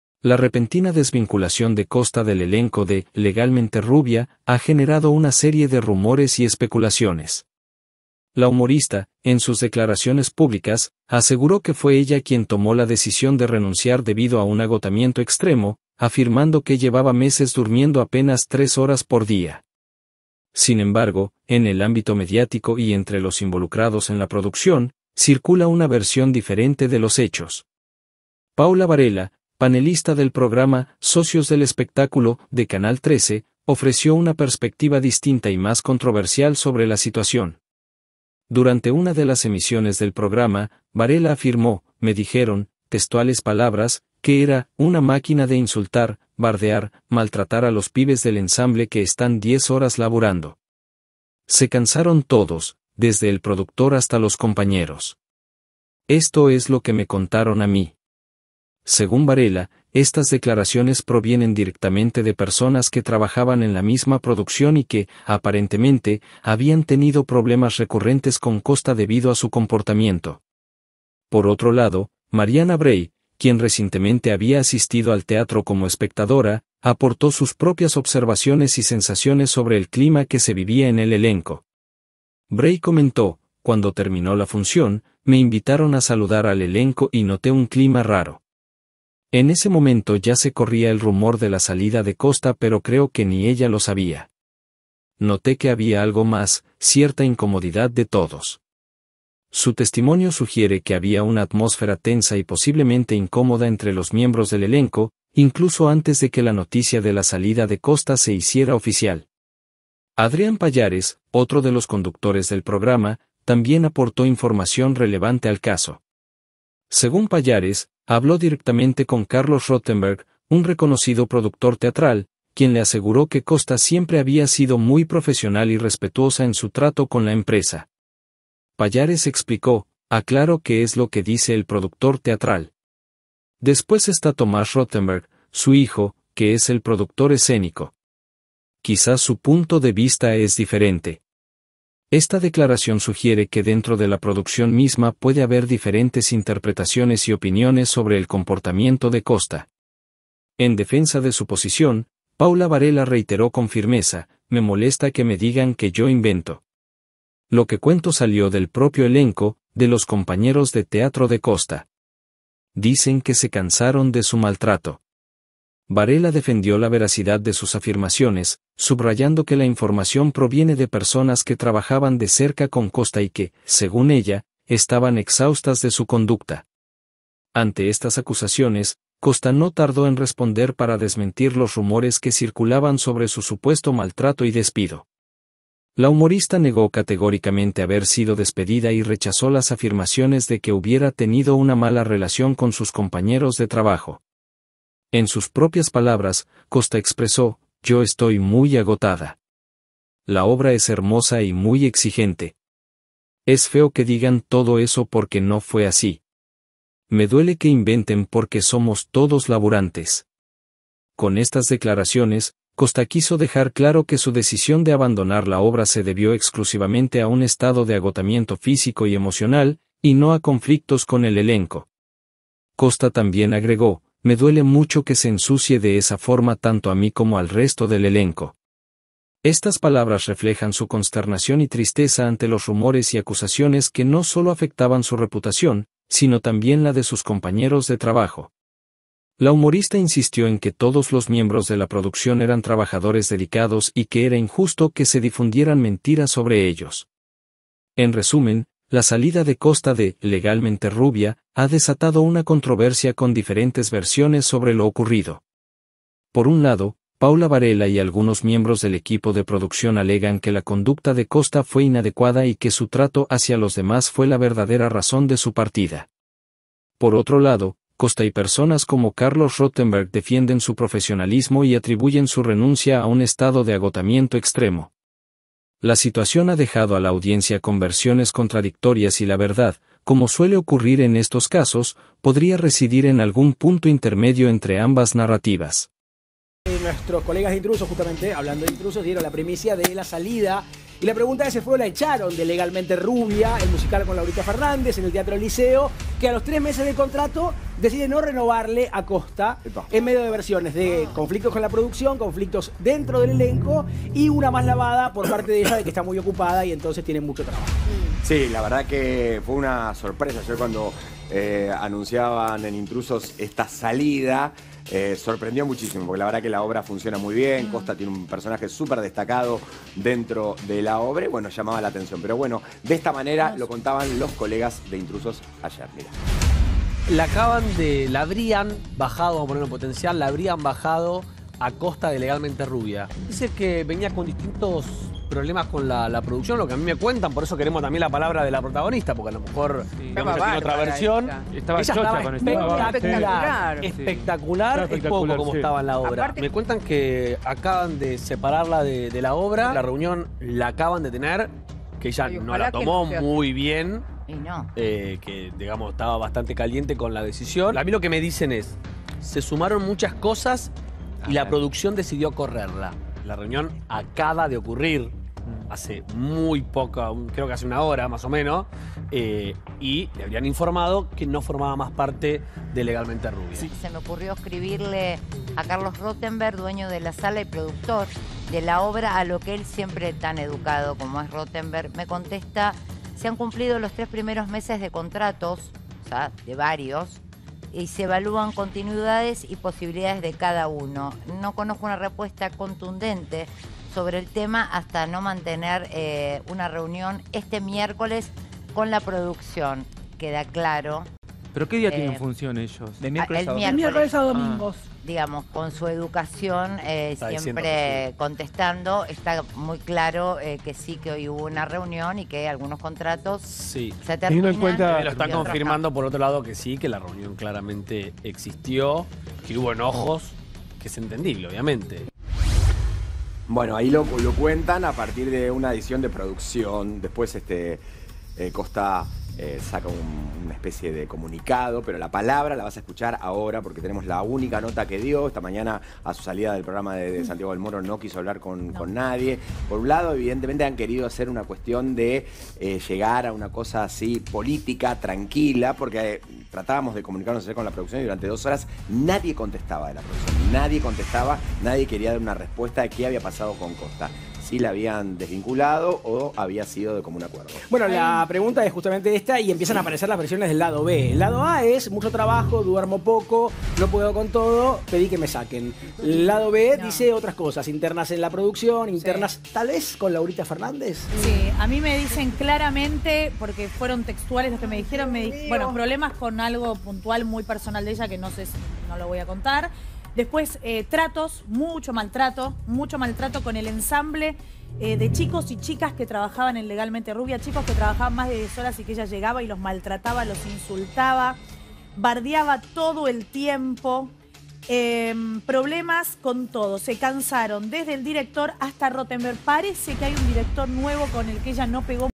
Se revelan nuevos detalles sobre desvinculación de Costa en el elenco de Legalmente Rubia
En "Socios del Espectáculo" revelaron detalles inéditos de lo que sucedía tras bambalinas y que hasta ahora, nadie conocía.